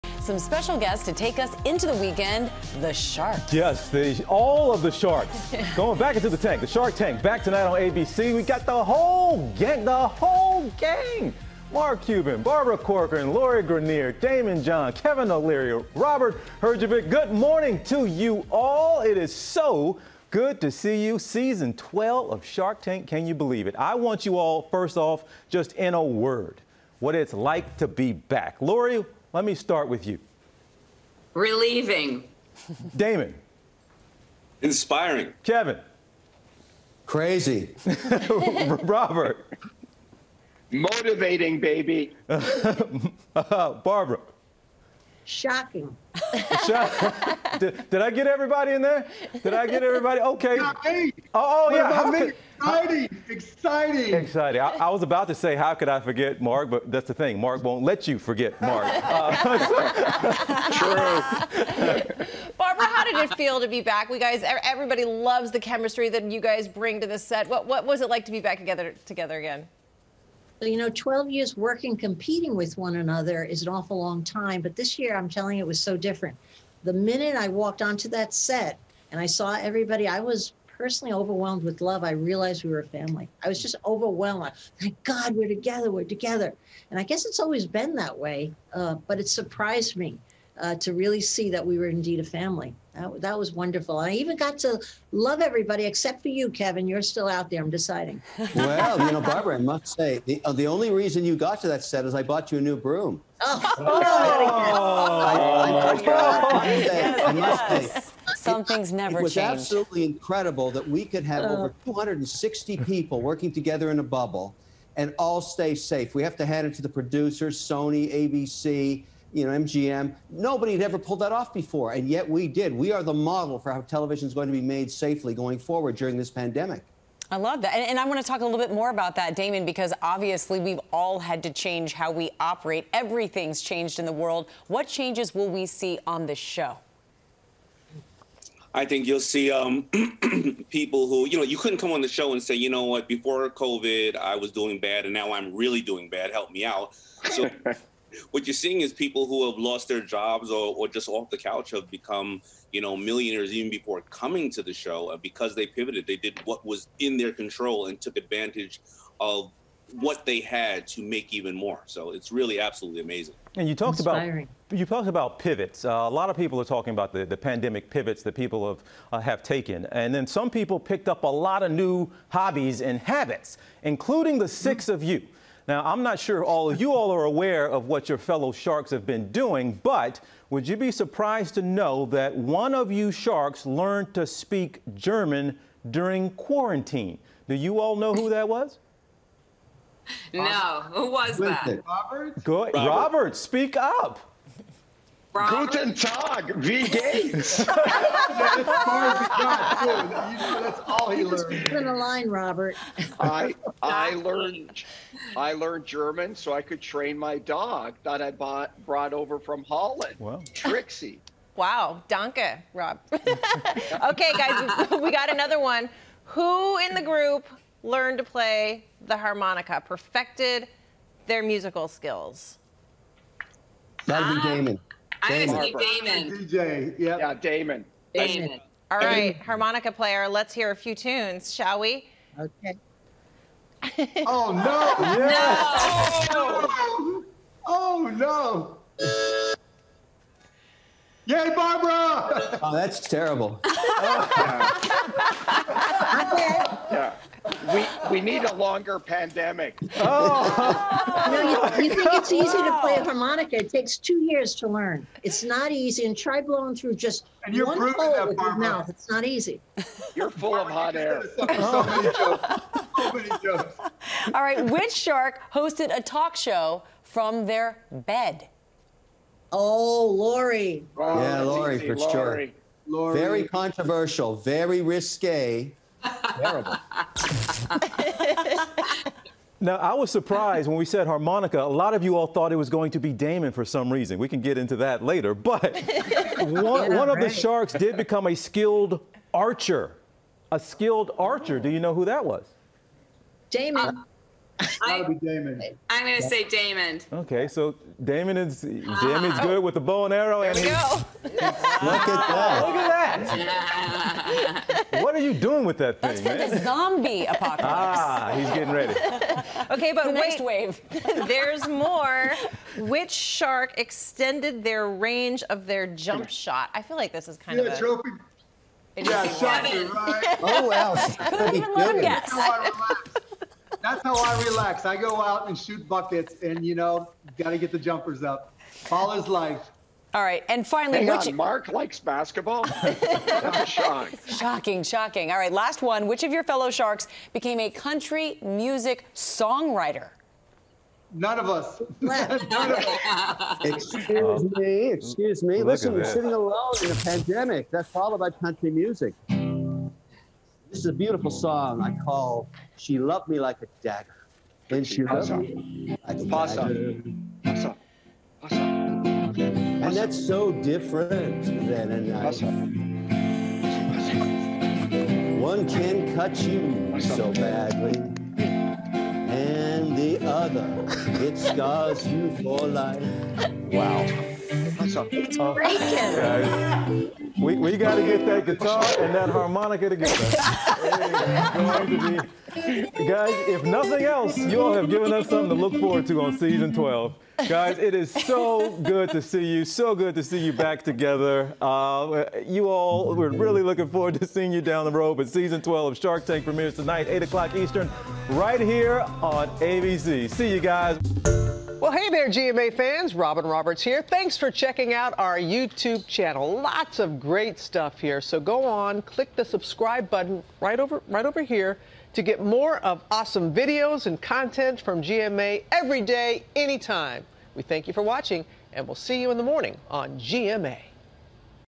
‘Shark Tank’ stars talk about show ahead of season premiere l GMA
Mark Cuban, Barbara Corcoran, Lori Greiner, Daymond John, Kevin O’Leary and Robert Herjavec share how they filmed the new season under strict COVID-19 safety guidelines.